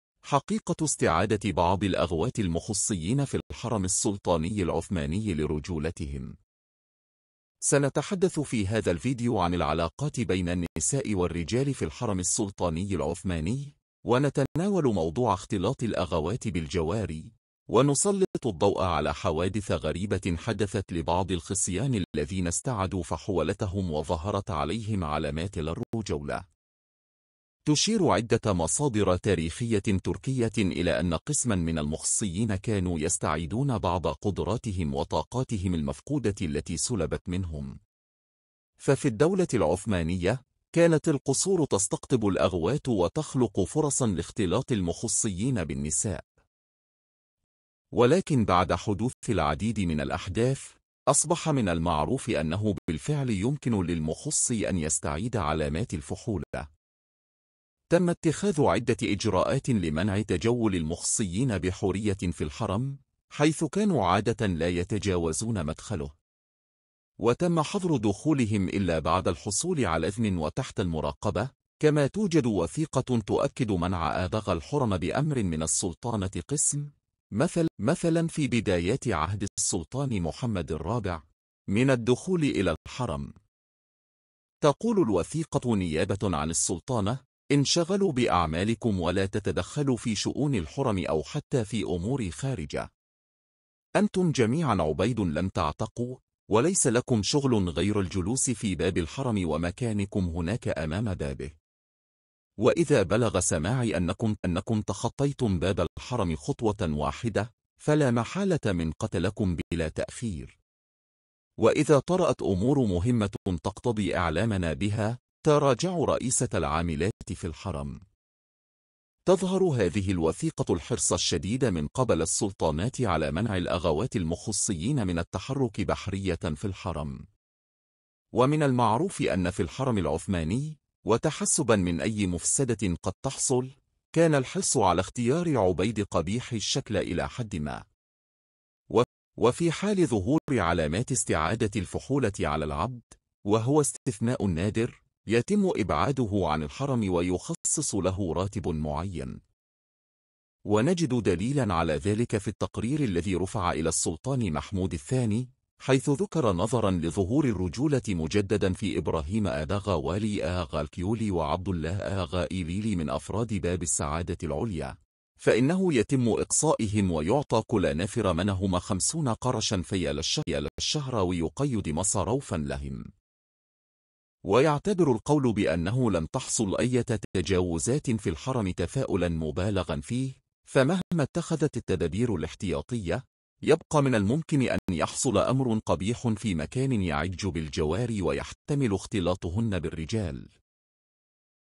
حقيقة استعادة بعض الأغـوات المخصيين في الحرم السلطاني العثماني لرجولتهم
سنتحدث في هذا الفيديو عن العلاقات بين النساء والرجال في الحرم السلطاني العثماني، ونتناول موضوع اختلاط الأغوات بالجواري، ونسلط الضوء على حوادث غريبة حدثت لبعض الخصيان الذين استعادوا فحولتهم وظهرت عليهم علامات الرجولة.